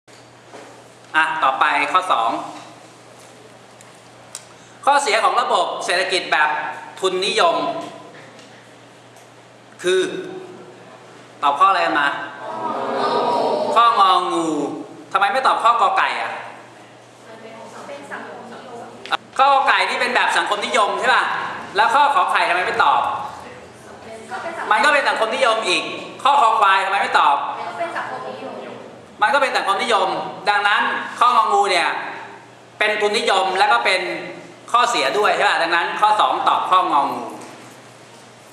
เฉลยแบบฝึกหัด ตอนที่ 1 ข้อ 2
Solution Microeconomics ความรู้ทั่วไปเกี่ยวกับเศรษฐศาสตร์ โดย อ.มานพ สีเหลือง สถาบัน Yellowการบัญชี